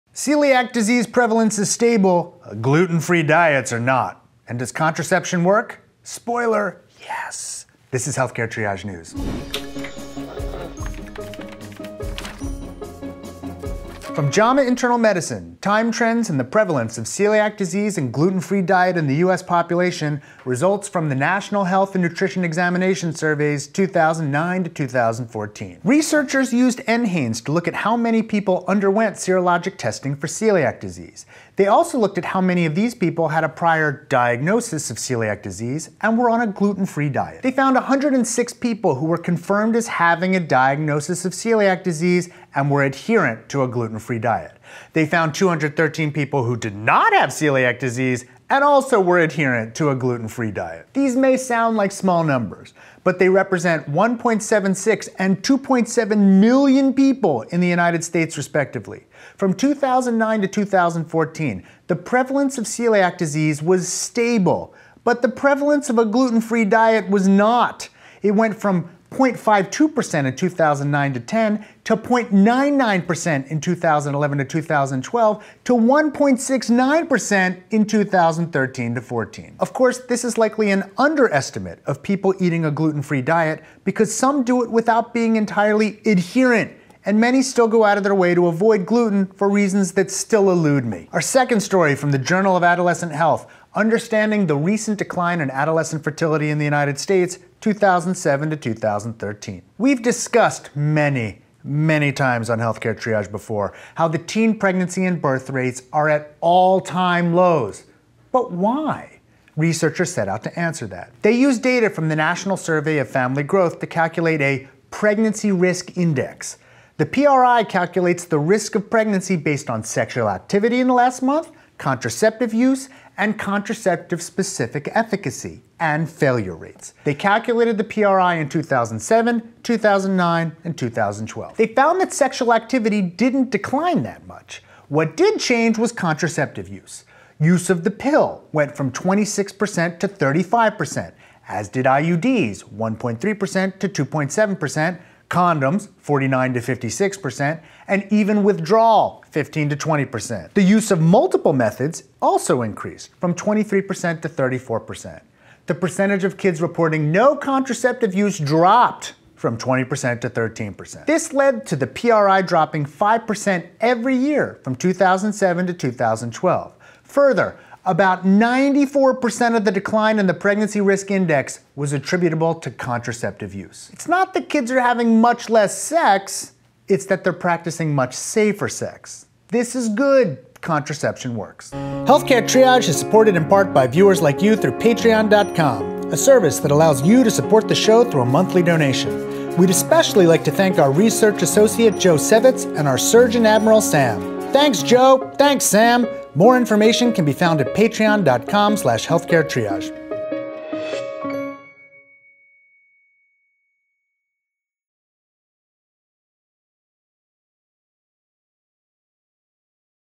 Gluten-Free Diets on the Rise, and Contraception Works
Celiac disease prevalence is stable; gluten free diets are not. And does contraception work? Spoiler… yes. This is Healthcare Triage News.

Those of you who want to read more can go